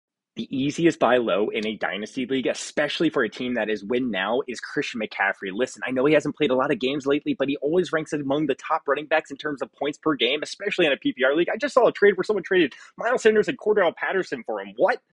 The Easiest Buy Low in Dynasty
The Easiest Buy-Low for a win-now team in a dynasty league is Christian McCaffrey. Yes, he only played 7 games last season and 3 games the NFL season before that, but he always ranks as one of the top RBs in PPG… Especially in PPR leagues.

I just saw a trade where someone traded Miles Sanders and Cordarrelle Patterson for CMC…

CMC is a great dynasty buy-low trade target!

I am sure the person that has CMC in your league is getting frustrated. See if you can buy low on Christian McCaffrey.